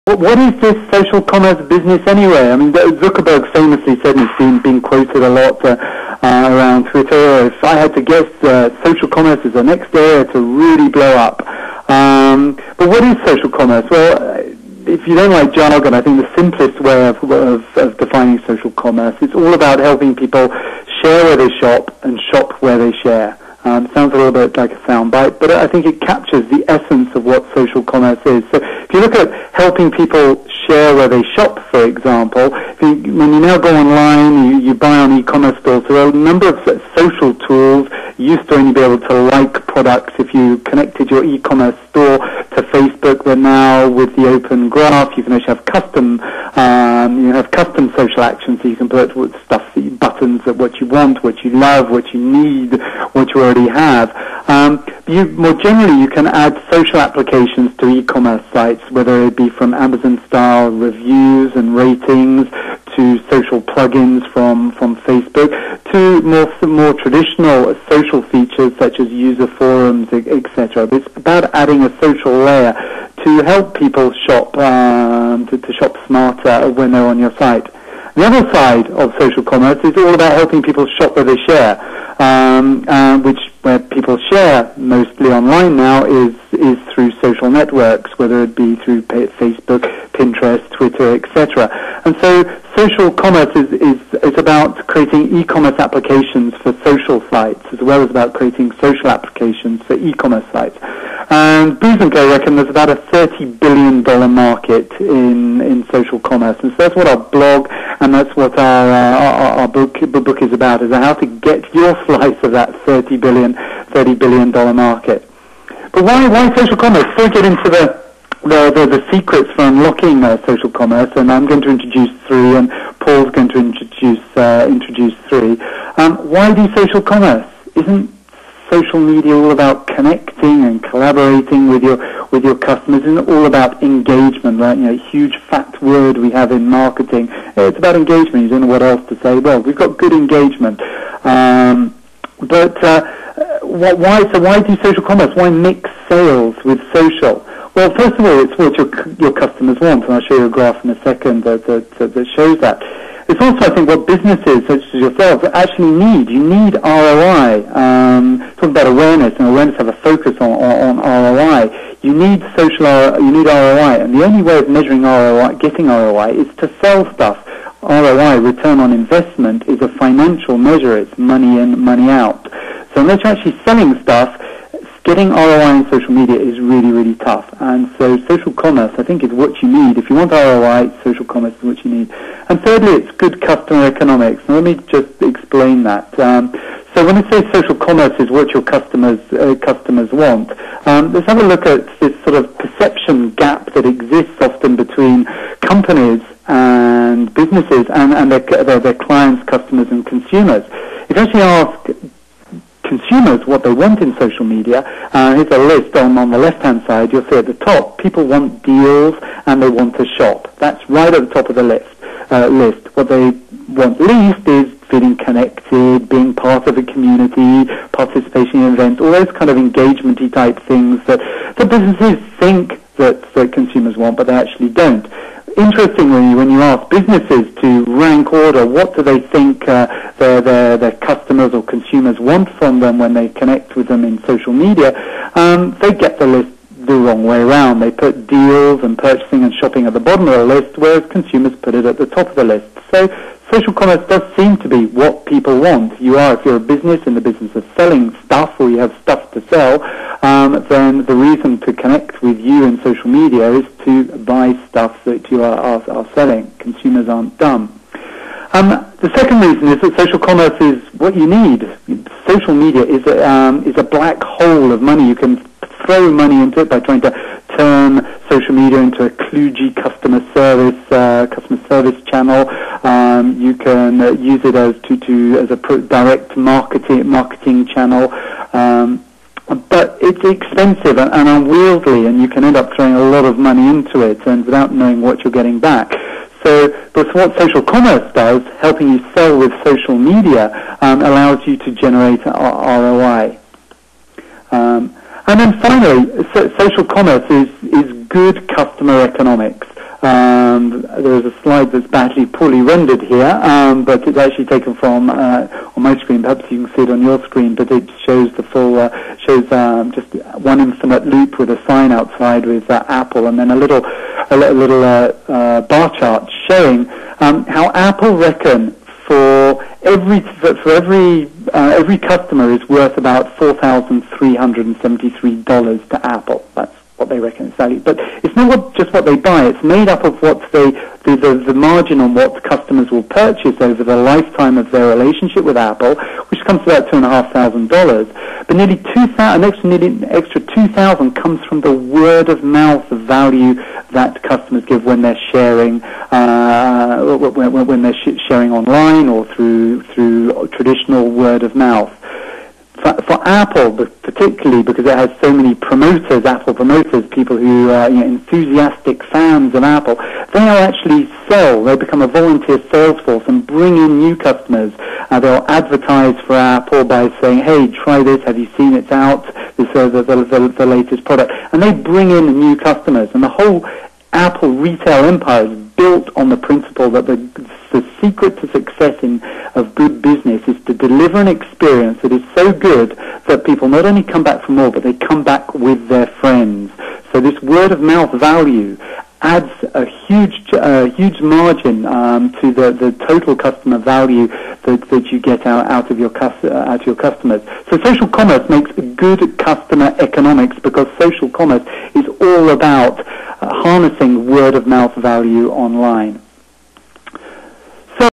What Exactly is Social Commerce
A clip from our webinar with Paul Marsden and Paul Chaney about Social Commerce